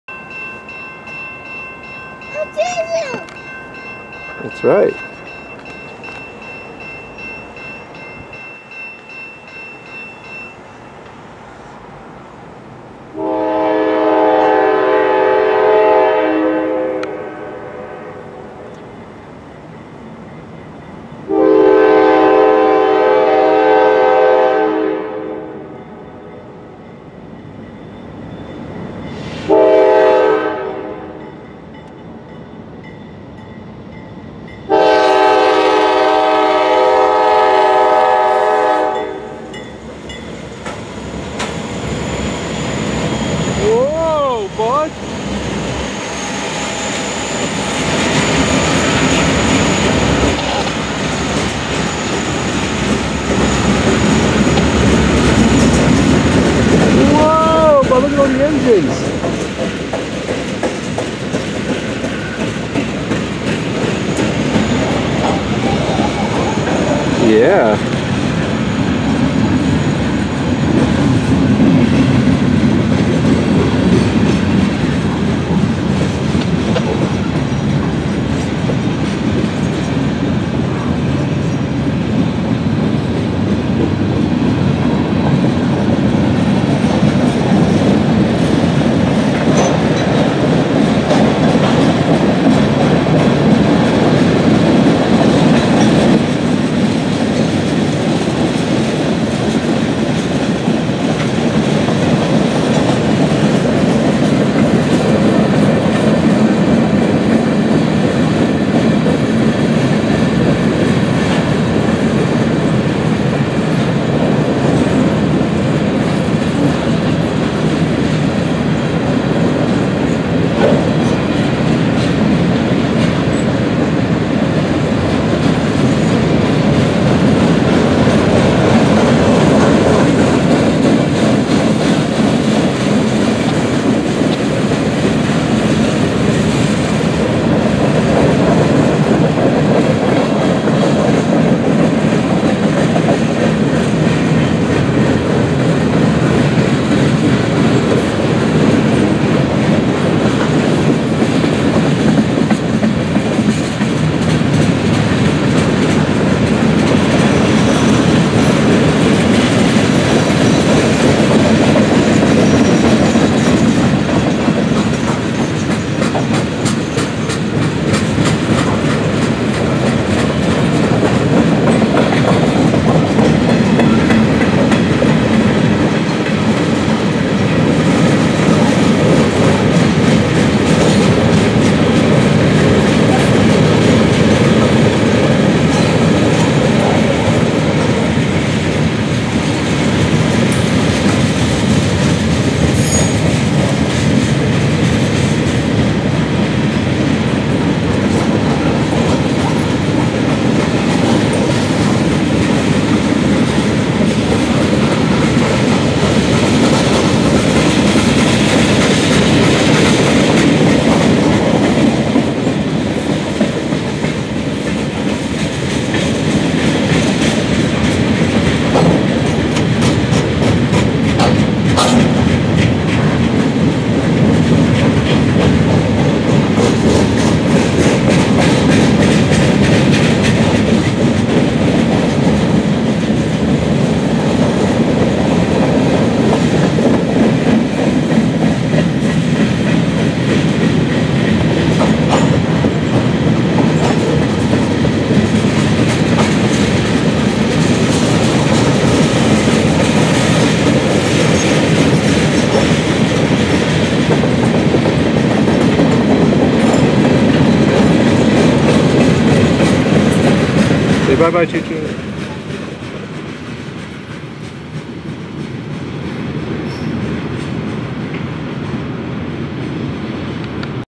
11 engine power move on mixed freight
Cought by surprise, this northbound was hauling 11 engines most probably for selkirk. Nice mixed freight in tow aswell.